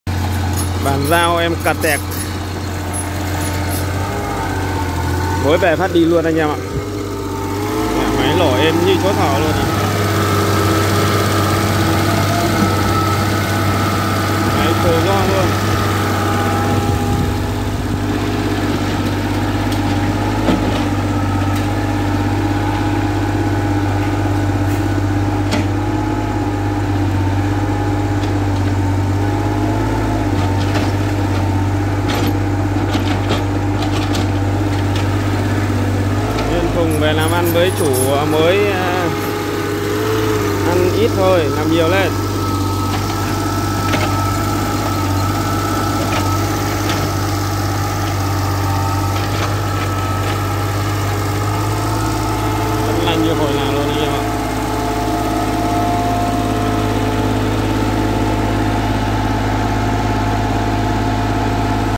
Bàn giao CAT 015 cho khách hàng
bàn giao máy xúc đào cat 015 cho khách hàng,
cảm ơn quý khách đã ủng hộ sản phẩm của chúng tôi, quý vị đang có nhu cầu về máy công trình xin liên hệ để biết thêm thông tin chi tiết